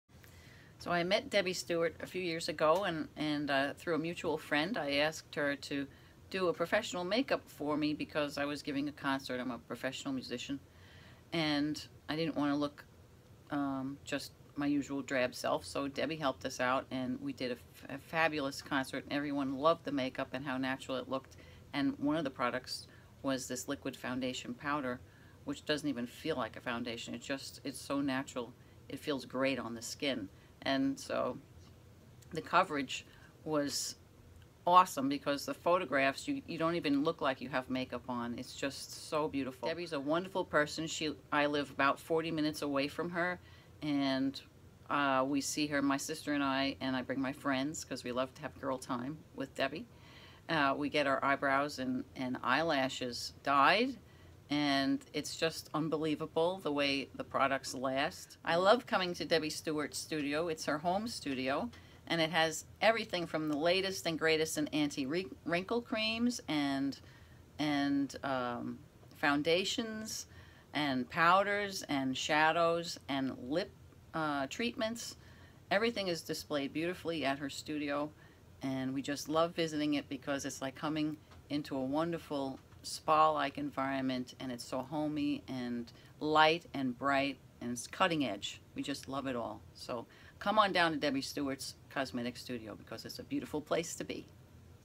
Lisa Testimonial
Lisa, a long time customer and friend of Debbi Stewart shares her views on her favorite Debbi Stewart products!